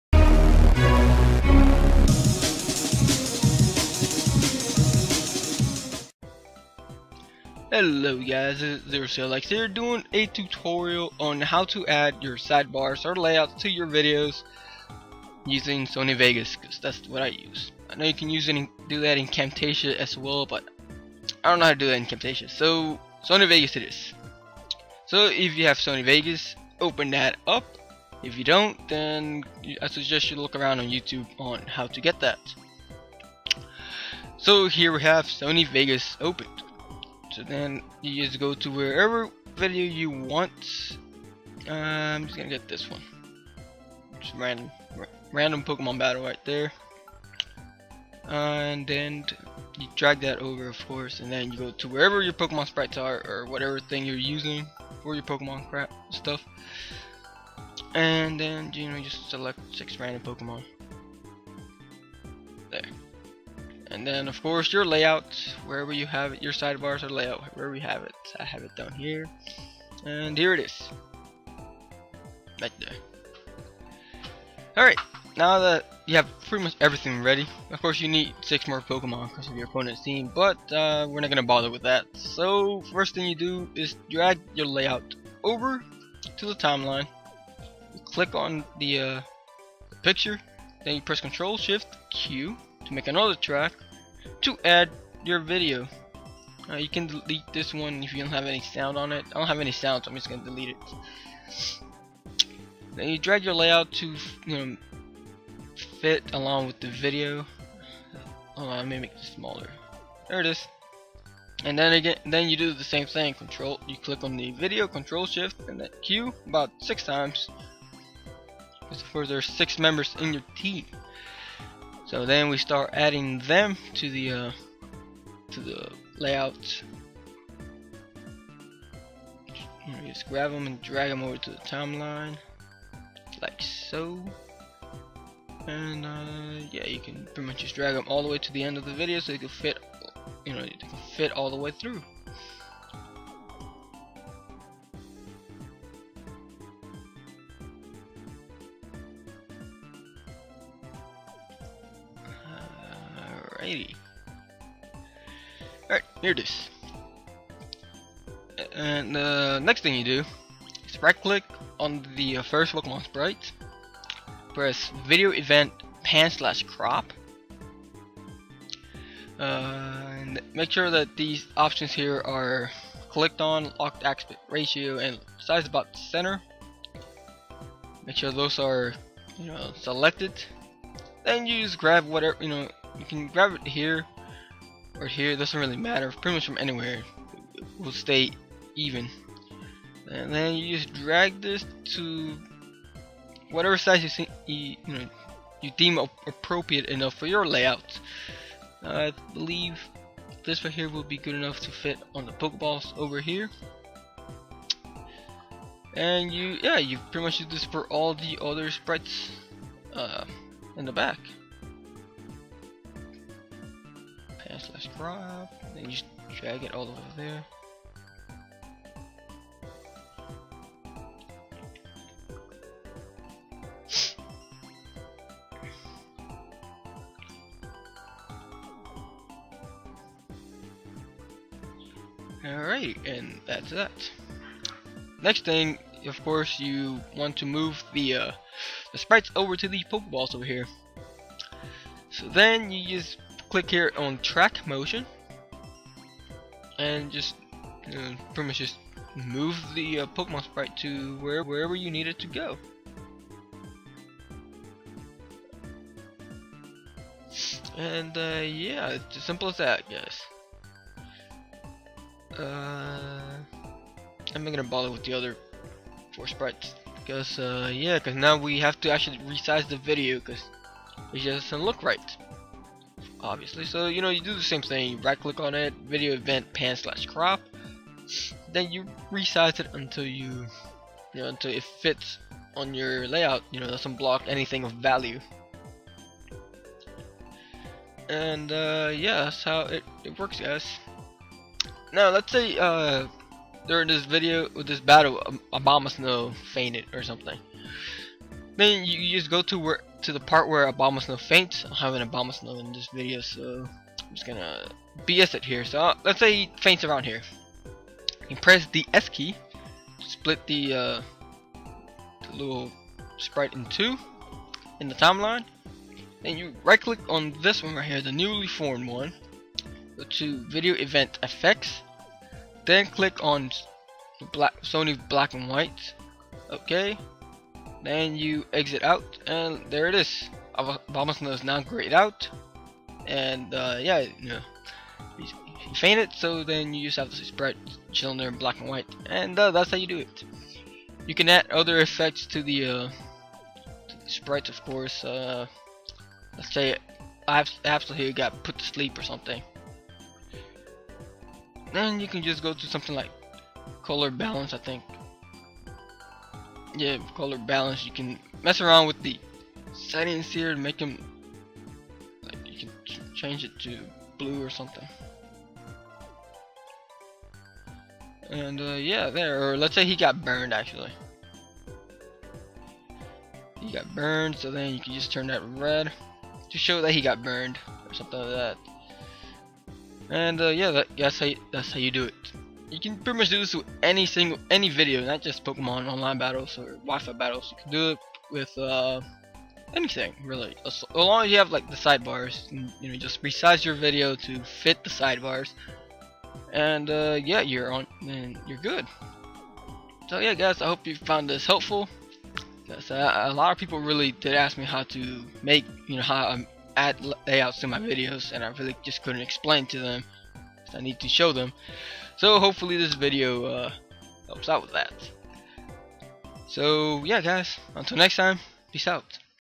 Tutorial - Adding Sidebars/Layouts to your Videos using Sony Vegas
A lot of people always ask me about this so yeah. Everything is explained in the video, so watch it :P